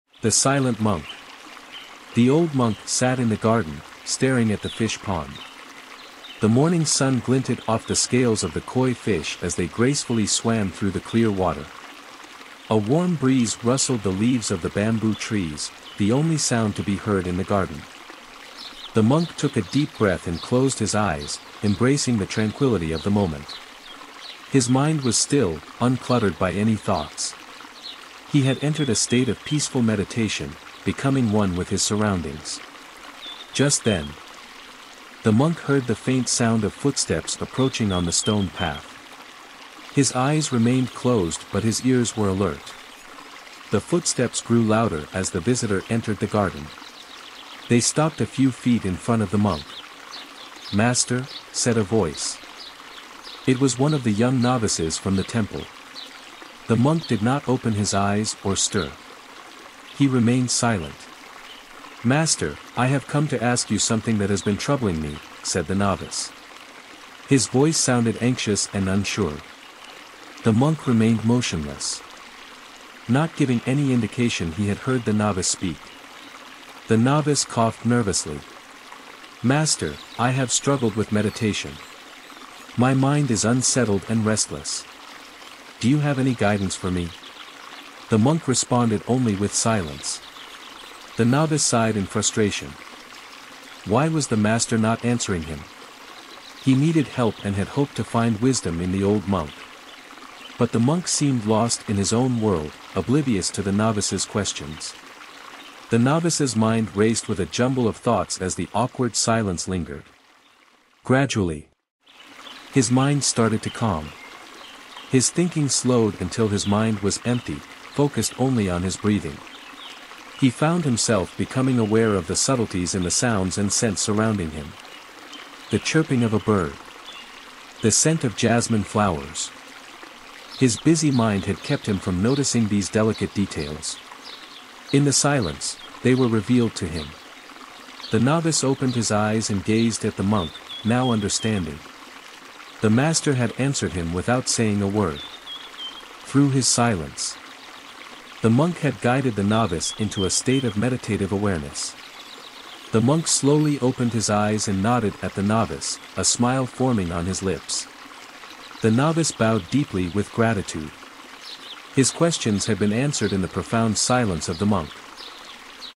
The Silent Monk - Zen Story
The Silent Monk - Zen Story: The old monk sat in the garden, staring at the fish pond. The morning sun glinted off the scales of the koi fish as they gracefully swam through the clear water. A warm breeze rustled the leaves of the bamboo trees, the only sound to be heard in the garden.

Together, let's embark on a path of enlightenment and inner harmony. 🌟